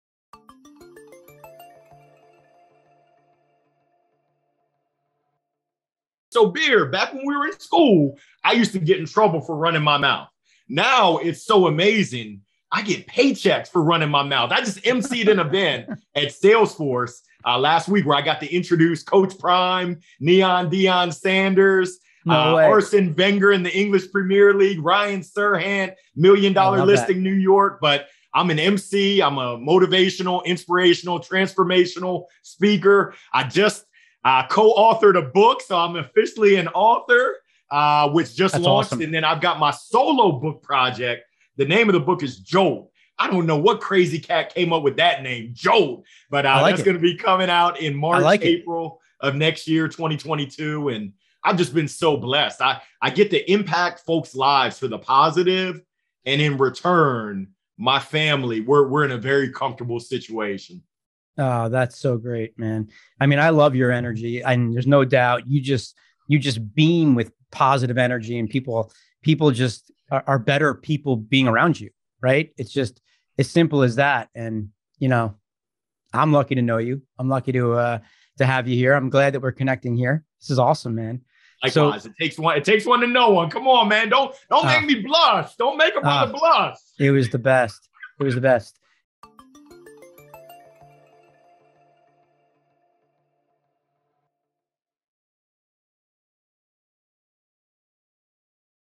Motivational Speaker Larry Long Jr: How Does a Mindset Coach Make Money? | the Power of Words
👉 Listen to the motivational speaker and sign up for the SurveyDetective VIP Waitlist to learn how to be successful in online marketing

Are you ready for some positive energy?!

I had the pleasure to chat with motivational speaker Larry Long Jr, an old friend of mine who played college baseball with me at the University of Maryland.

Back then, we competed against each other. If we hadn’t, we could have been best friends since we have similar personalities - we both empathize and want to help people. Bonus: We are talkers - we could talk all day long!

And Larry has found a way to make money out of these traits. As a transformational speaker and a mindset coach, he helps massive companies unlock their employees’ full potential and impact lives for the positive.

Or as Larry likes to put that…

“When we were in school, I used to get in trouble for running my mouth. Now I get paychecks for running my mouth.”

And today, he is a treat for us!

Tune in and find out how to develop a positive mindset to be successful in anything you do, why it is so important to be surrounded by good people, and what the mamba mentality is.

- Visit Larry Long Jr’s website,
- Connect with Eric!